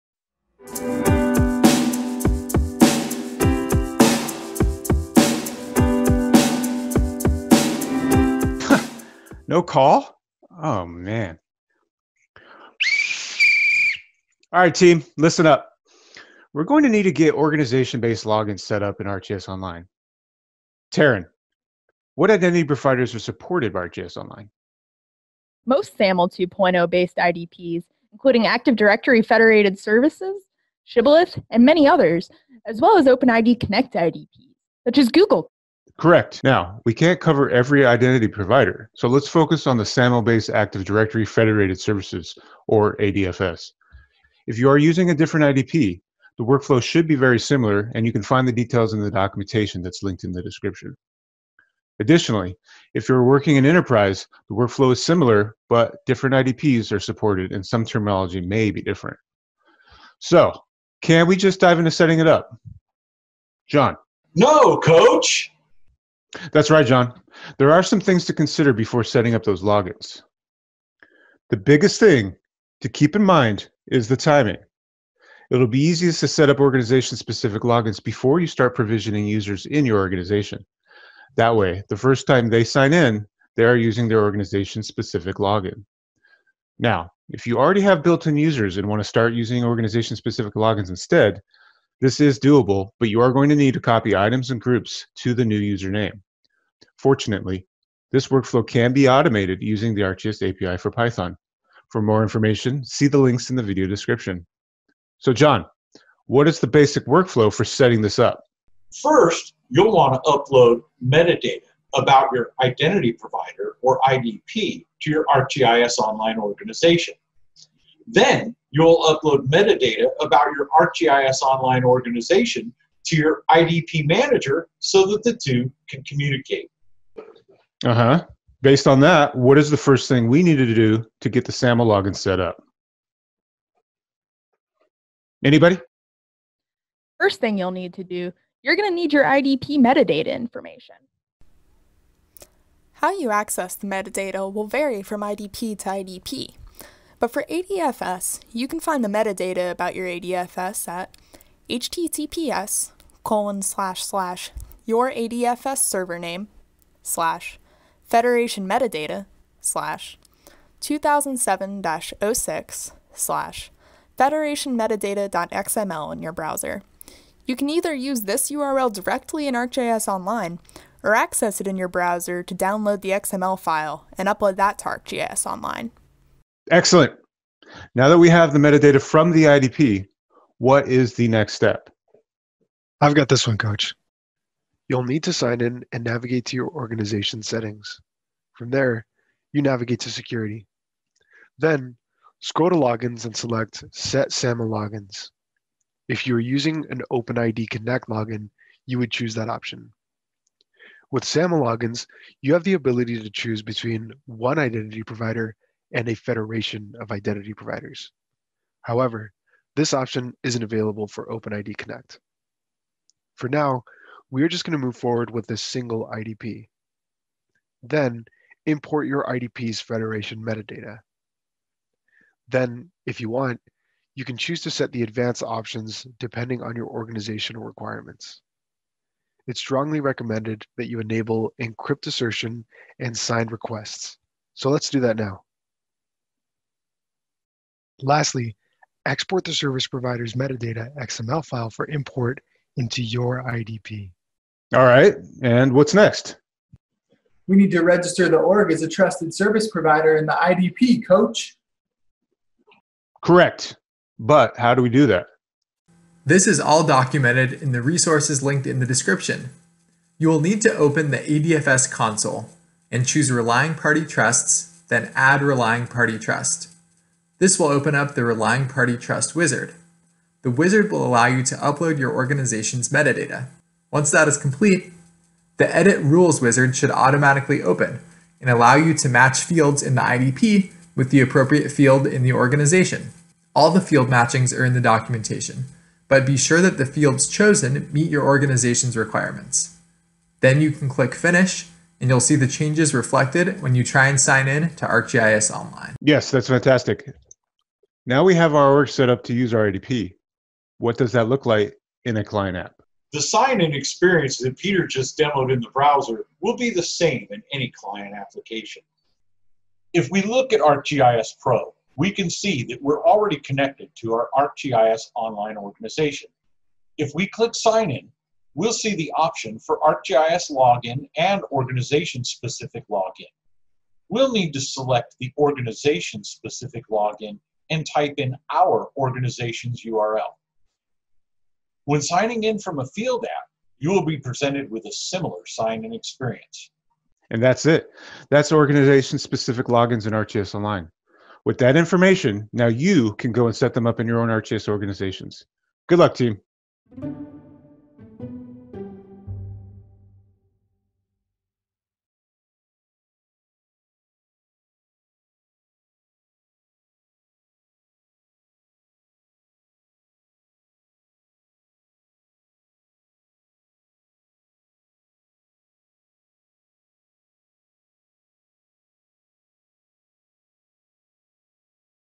Setting up Organization Specific Logins in ArcGIS Online
Learn how to set up Active Directory Federated Service (AD FS) logins for ArcGIS Online. This video reviews how to get AD FS metadata, how to set up ArcGIS Online to use AD FS, and how to configure relying party trusts in AD FS.